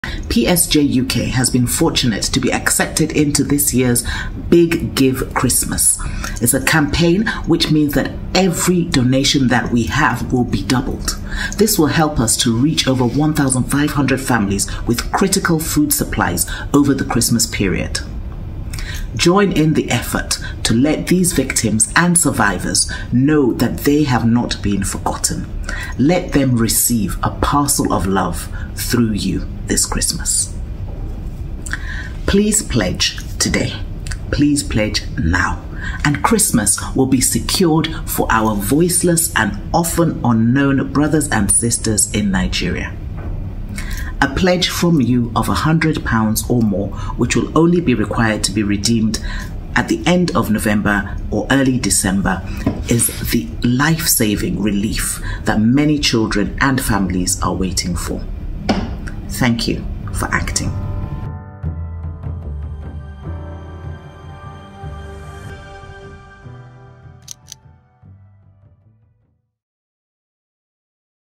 PSJUK BIGGIVE CHRISTMAS - DOUBLE IMPACT!
By pledging or donating, you will be supporting victims with food items such as rice, maize, millet, beans, yam, potatoes, and tomato paste, plus a few Christmassy treats in quantities enough to relieve over 1,500 families in Nigeria's middle belt from hunger this Christmas.